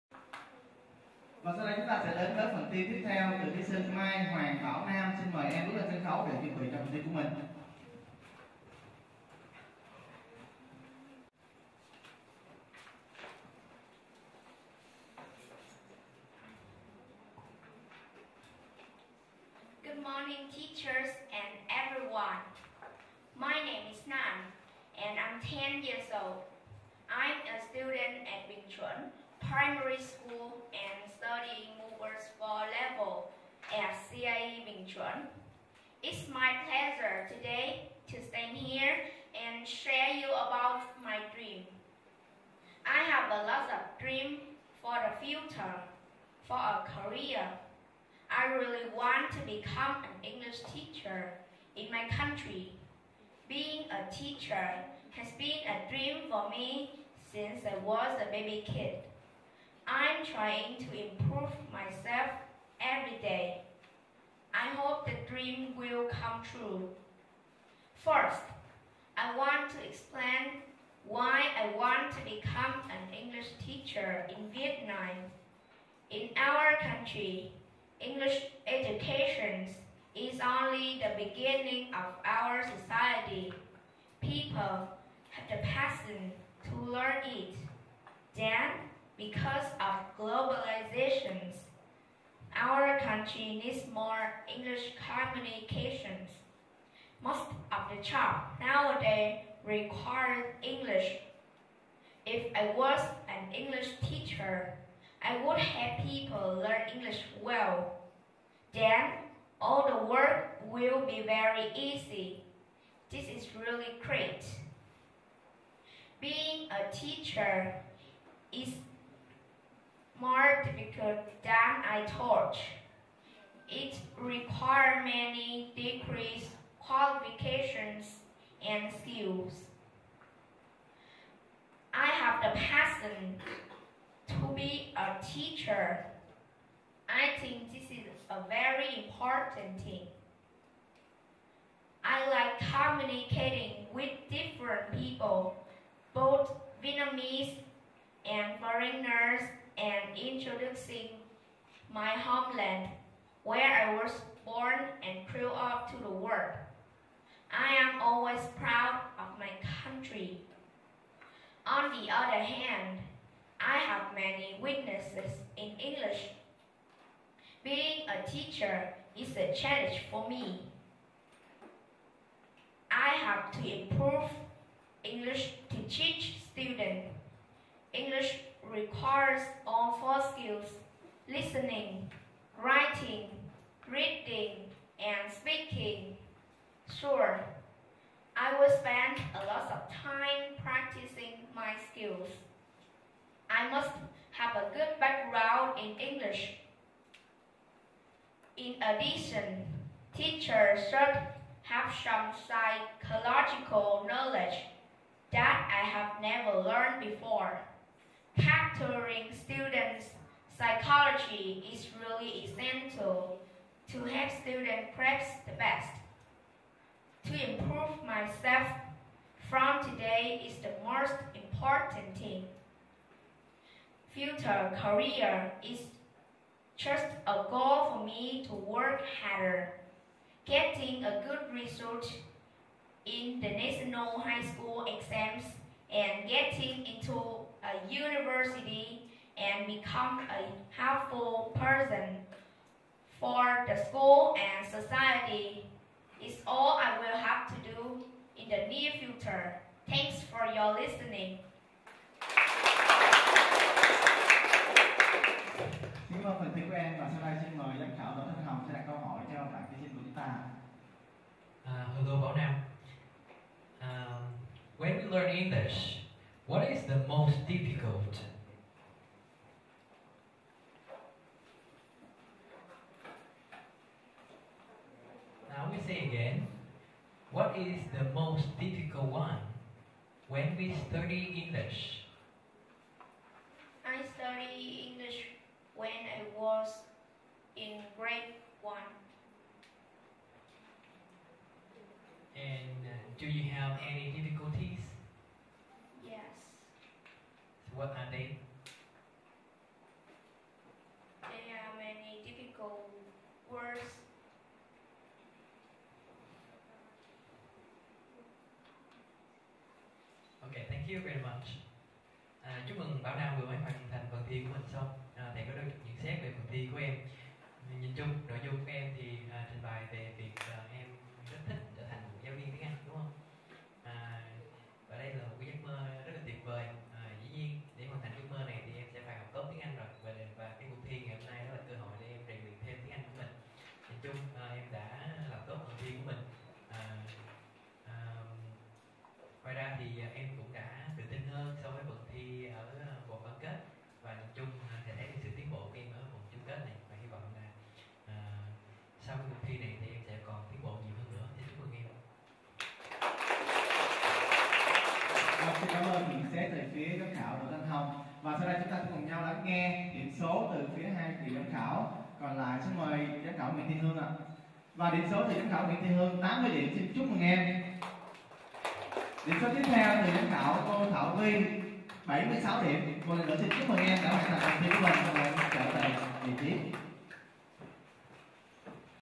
[SPEAK UP 2022 - TOP 10] MAI HOANG BẢO NAM
Vòng Chung kết cuộc thi nói Tiếng Anh dành cho học sinh tiểu học - Speak up 2022 - Chủ đề "I draw my dream" - 05/7/2022